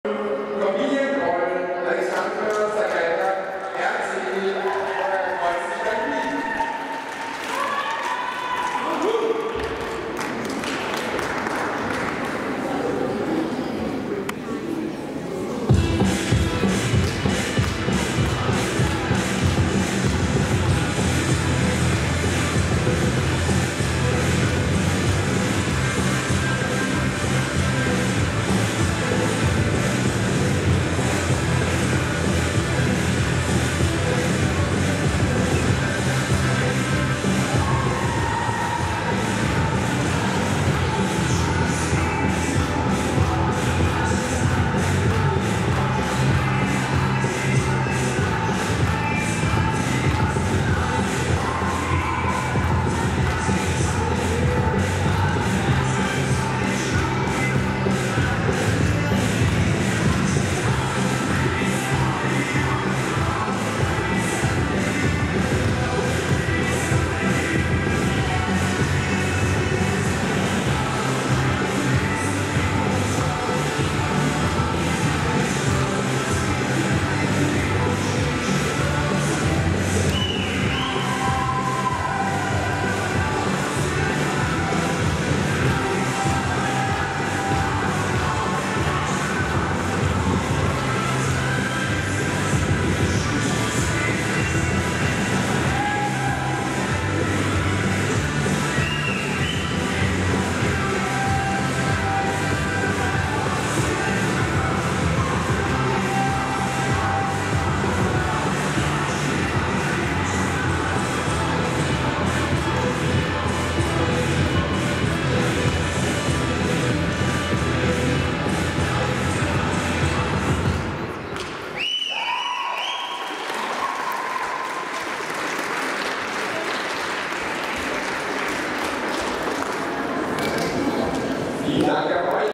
Acrobatic Rock'n'Roll Vienna Cup B-Class 2018-03-24
Korbinian - Alexandra, SPORTUNION Rock'n'Roll Club Kreuzenstein Wien, ÖRBV member, 2. place.